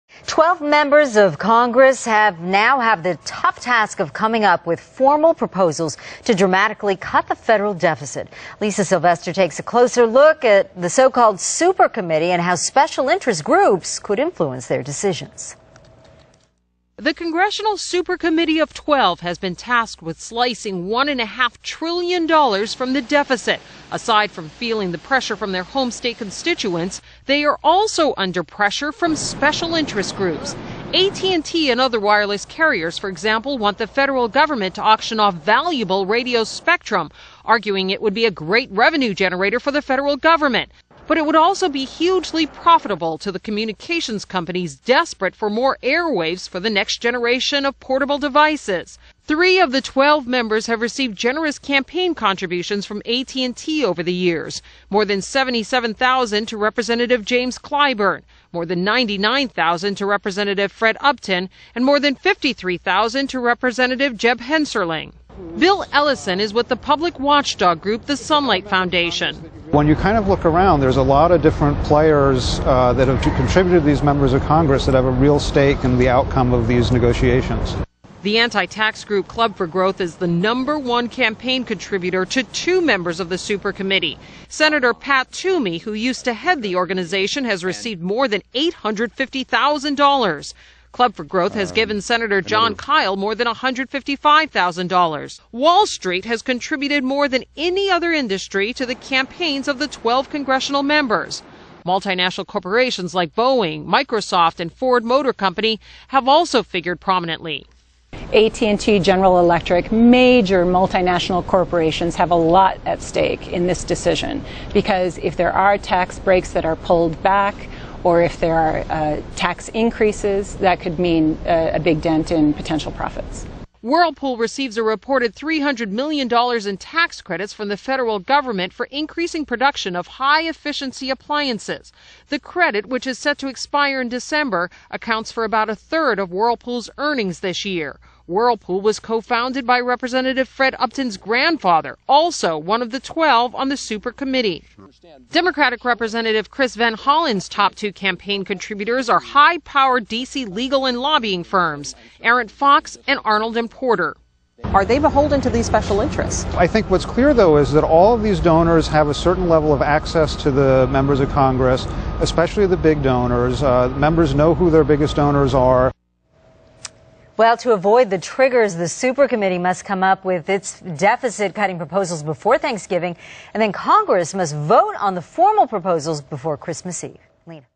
KCAL-TV -- Bill Allison -- Super Committee Conflicts of Interest
Lisa Sylvester reports for KCAL in Los Angeles, CA from Washington, DC on the lobbying of the Super Committee and interviews Bill Allison from Sunlight Foundation.